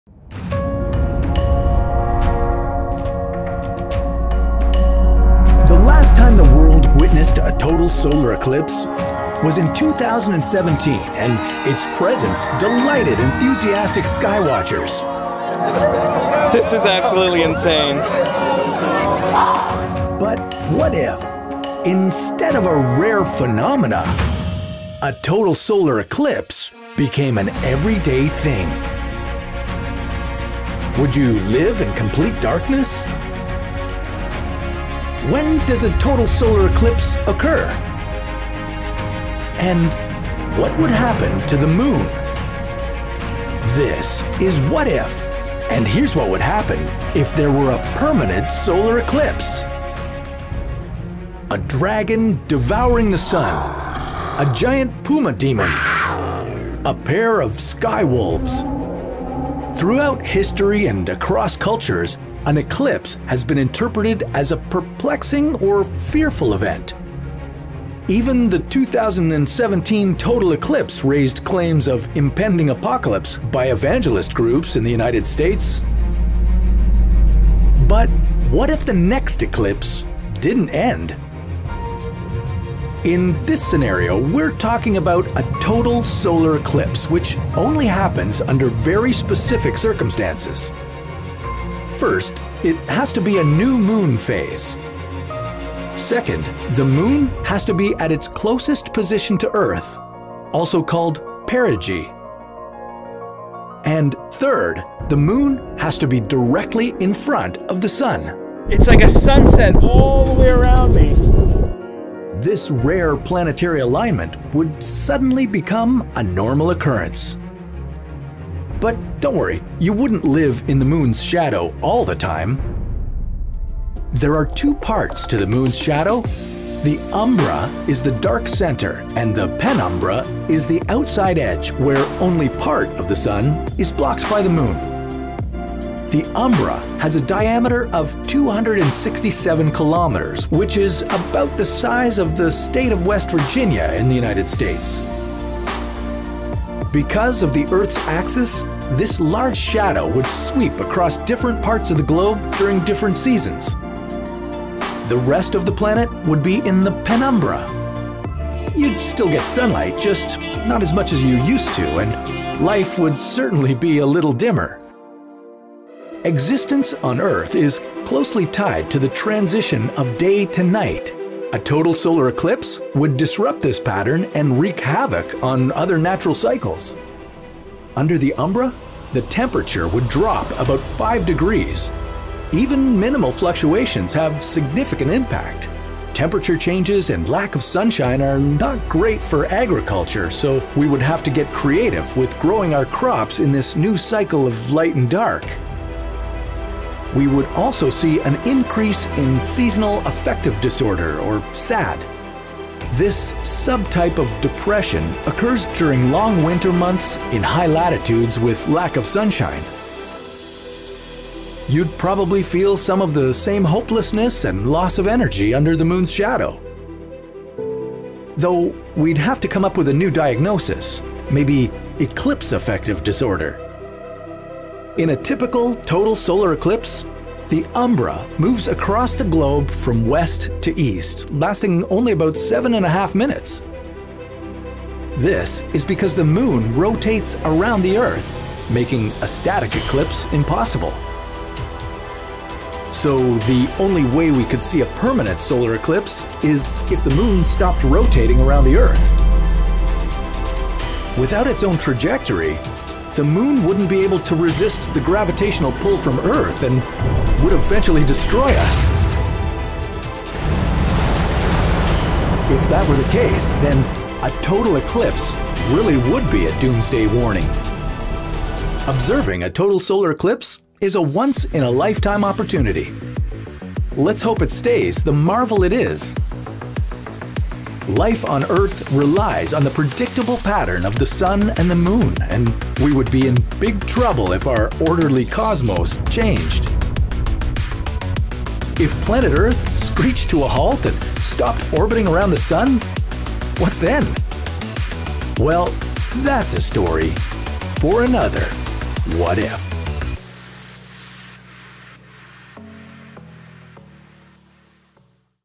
What If There Were a Permanent Solar Eclipse?
What if, instead of a rare phenomena, a total solar eclipse became an everyday thing? Would you live in complete darkness? When does a total solar eclipse occur? What would happen to the Moon?

Can you translate this episode into another language? Add subtitles and we will link your YouTube channel in the description